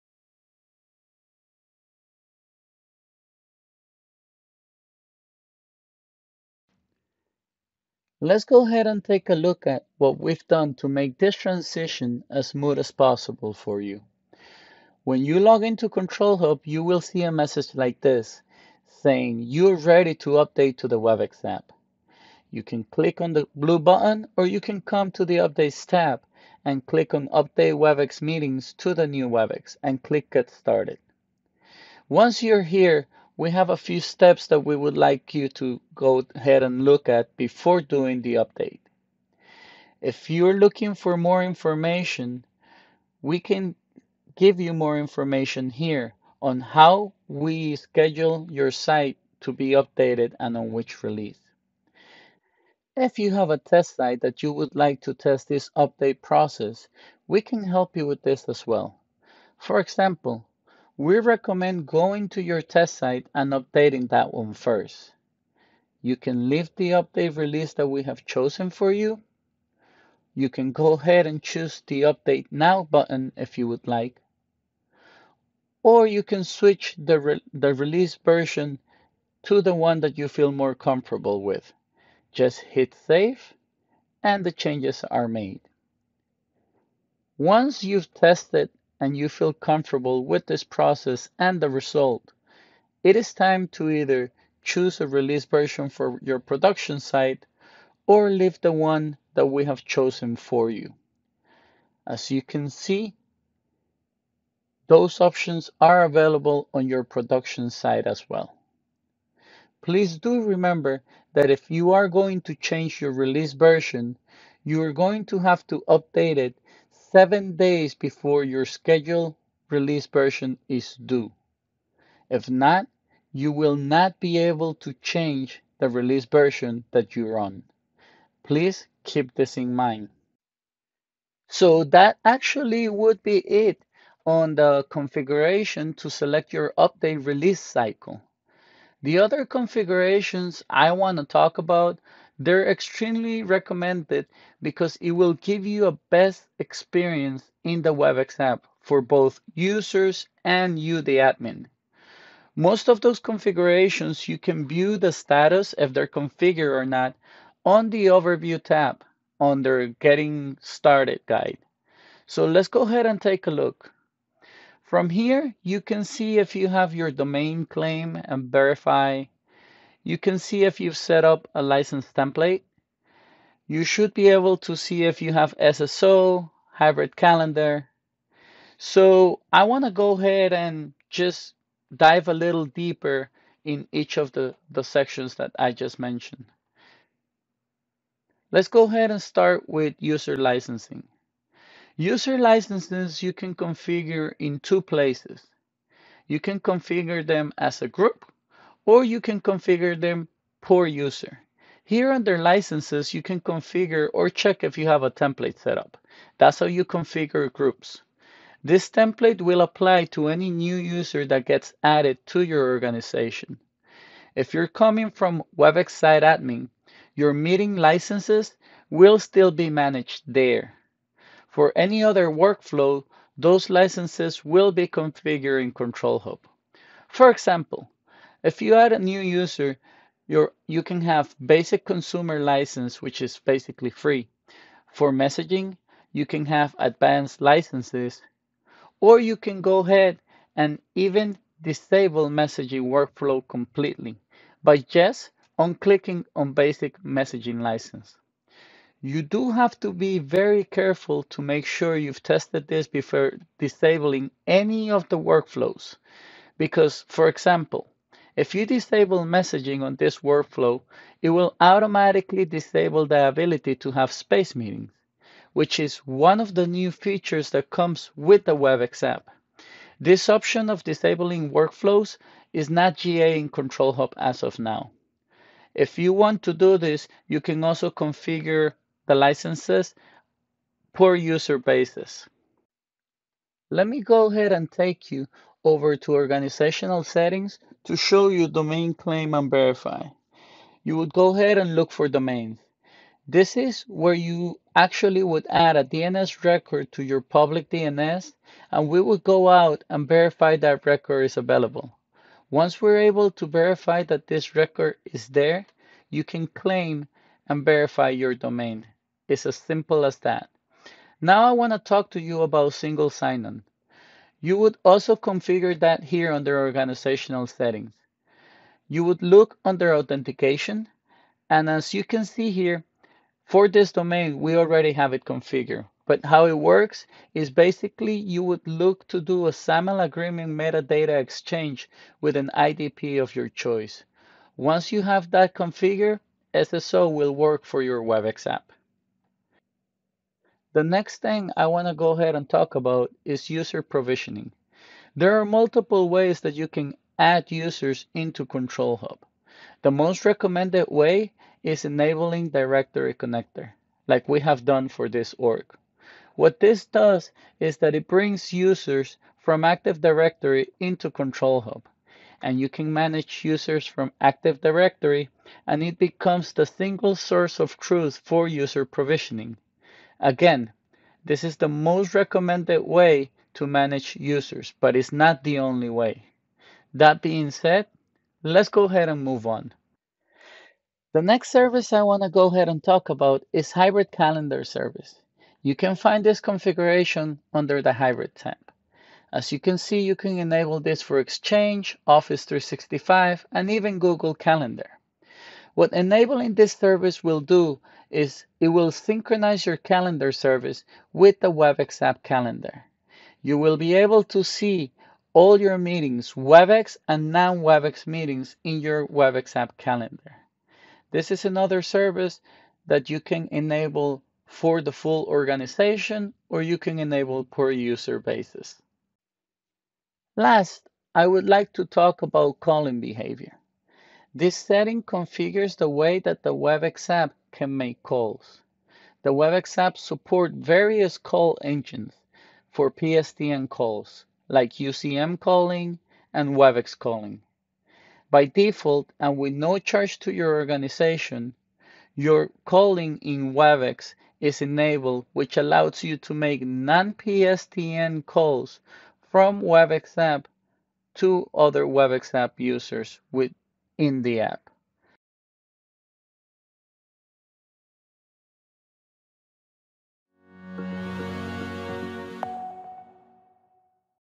Webex App Introduction to Control Hub
This video provides an introduction to CH in order to prepare for the update from Webex Desktop Meetings Client to the Webex App.